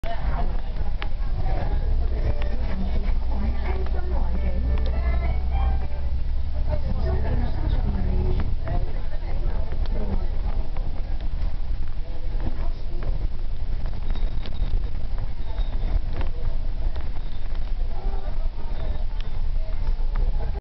KMB Route 18 tour on October 14, 2006 part 1
another bus tour on different routes in Hong Kong